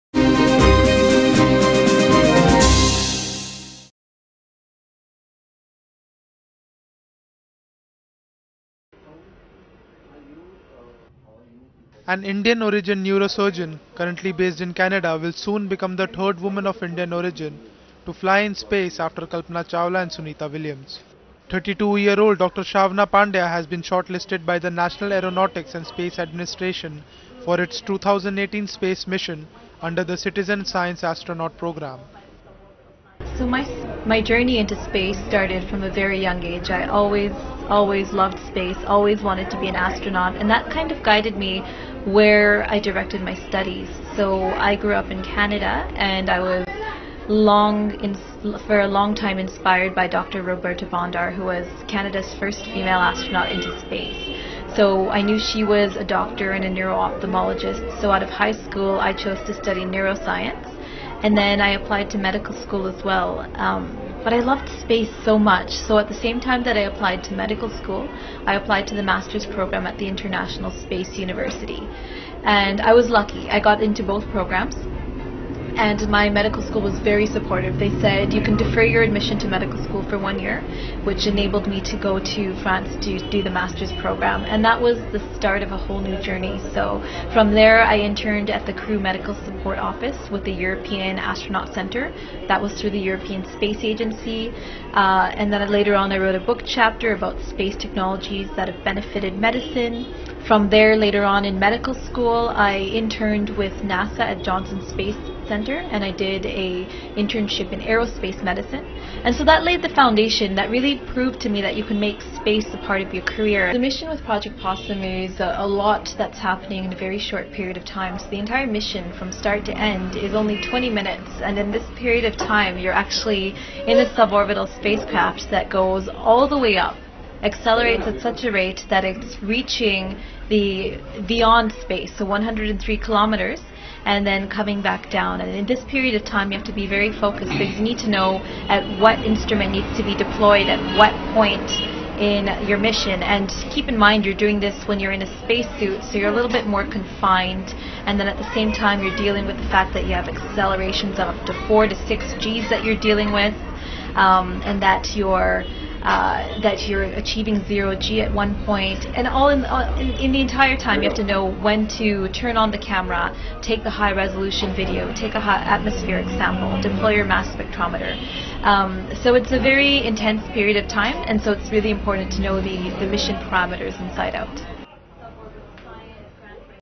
NASA to send Indian origin woman Dr Shawna Pandya in space | Oneindia News
Indian-origin neurosurgeon, Dr Shawna Pandya currently based in Canada, will be the third woman of Indian origin to fly in space after Kalpana Chawla and Sunita Williams. 32-year-old Dr Shawna Pandya has been shortlisted by The National Aeronautics and Space Administration (NASA) for its 2018 space mission under the Citizen Science Astronaut programme. Dr Pandya, who was born in Canada, is a general physician with the Alberta University Hospital and has her roots in Mumbai, where her grandmother currently lives in Mahalaxmi area. Besides, she is also an opera singer, an international champion in taekwondo and has also done her training in Muay Thai with a Navy SEAL, as per her bio-data.